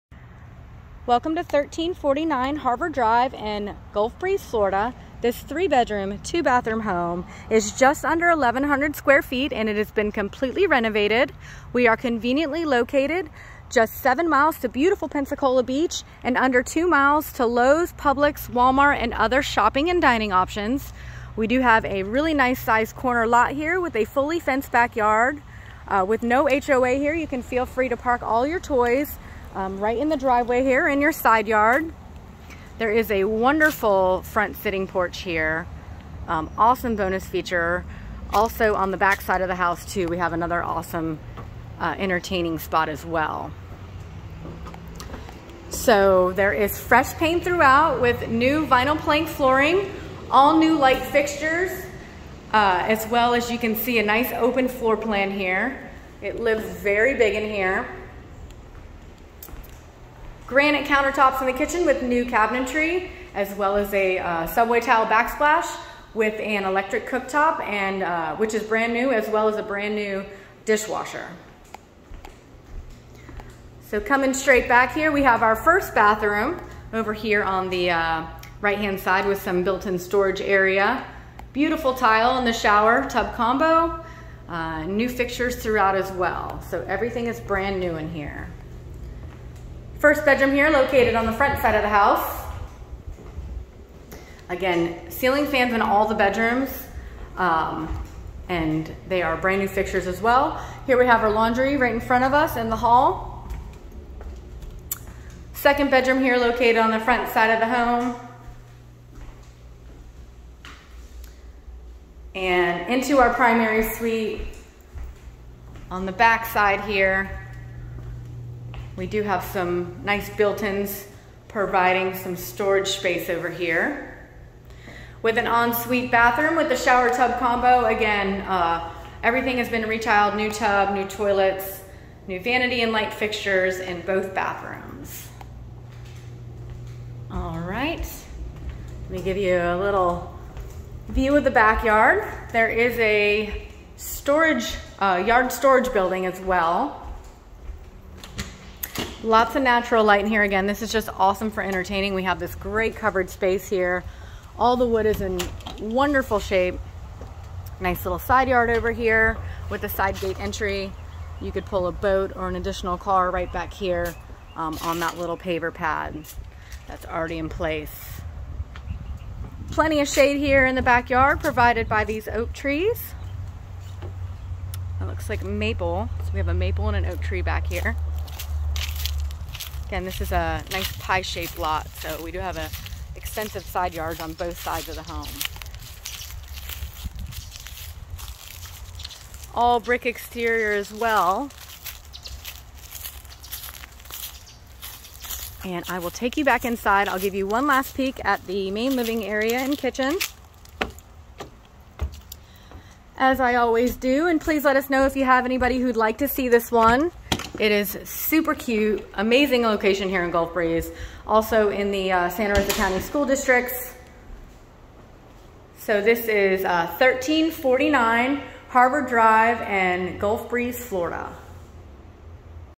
1349 Harvard Drive Gulf Breeze, Florida 32563
Home for sale Gulf Breeze, Florida. Offered at $290,000. Conveniently located in Gulf Breeze, this completely renovated bungalow offers two wonderful covered decks, a BRAND NEW ROOF and situated on a large corner lot. With new vinyl plank flooring throughout the entire home, you'll also note all new fixtures including new ceiling fans in all bedrooms, fresh paint, new doors and completely renovated bathrooms. The kitchen has all new cabinetry with a brand new electric cooktop and dishwasher with granite countertops and subway tile backsplash. Other mentionable features are keyless entry, spacious fenced back yard with mature trees, yard building and all brick exterior. With no HOA you can feel free to park your boat or RV right in the driveway. Located 7 miles to beautiful Pensacola Beach and less than 2 miles to Lowes, Publix, Wal-Mart and other dining and shopping options. 1 Mile to the Oriole Beach Public Boat Launch and located in Santa Rosa County School District.

Listing Agent: Travis Talley
The Talley Group